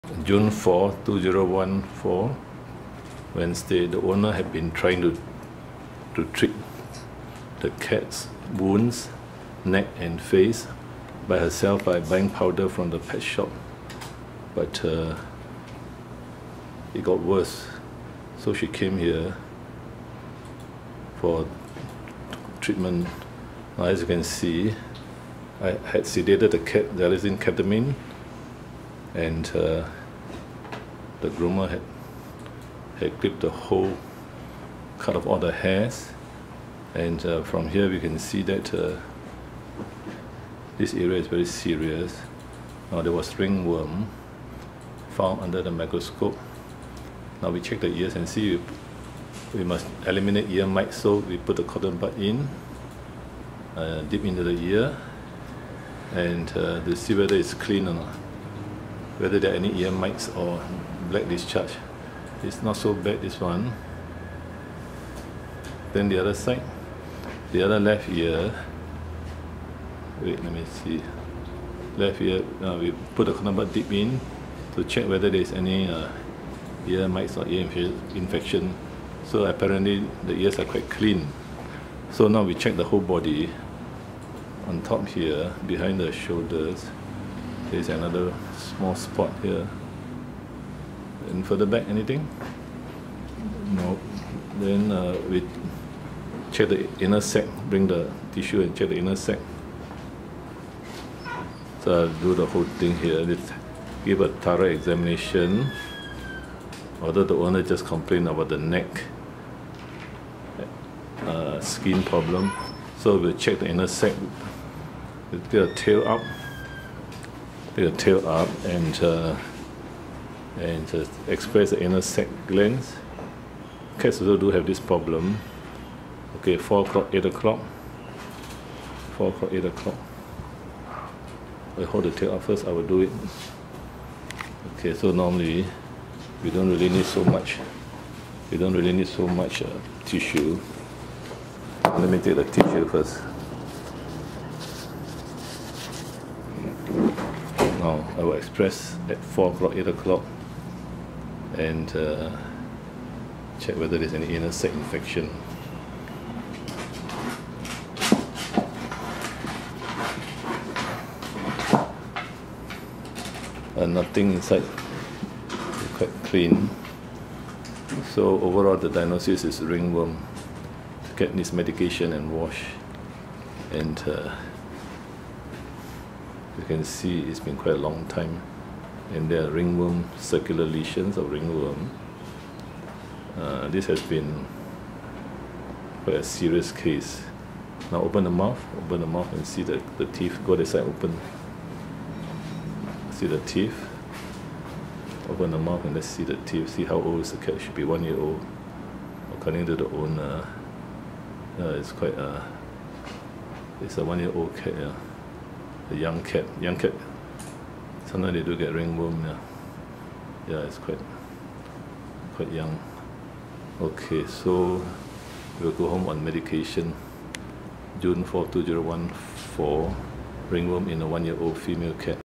A cat has itchy ringworm
Wed, Jun 4, 2014 A cat has neck ringworm

Self treatment by the owner was ineffective. The cat was sedated to give a proper clipping of the coat as cats dislike hair cuts. I expressed the anal sacs. No impacted anal sacs. Treatment is given. Review after one month is best.